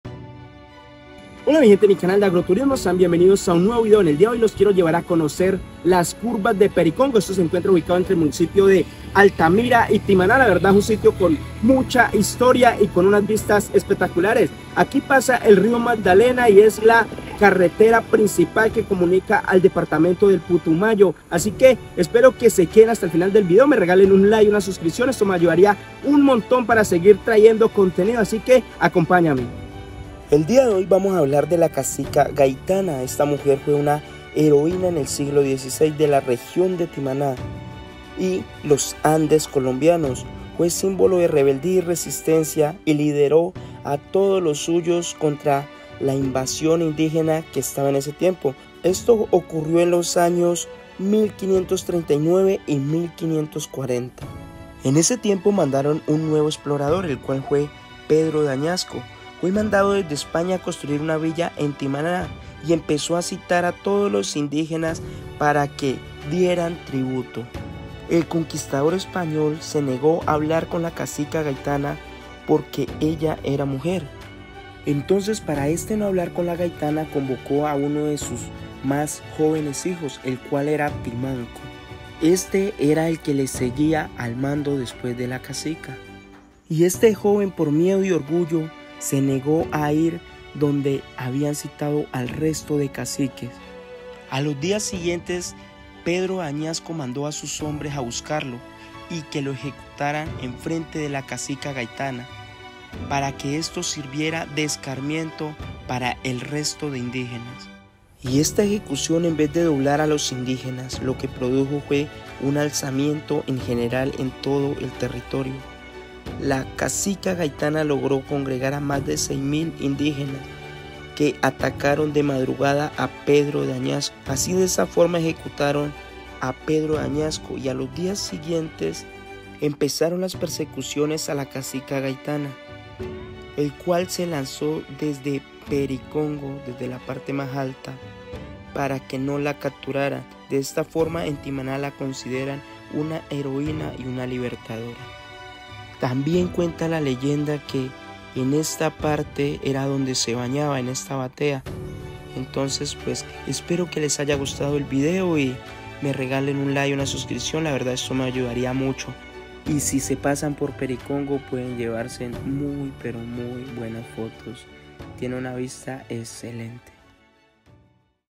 Pericongo
*En este video los quiero llevar a conocer un lugar con mucha historia, el cual se llama pericongo lugar turístico.

*Activa también las notificaciones para los nuevos videos.

*Gracias por su apoyo en mi canal.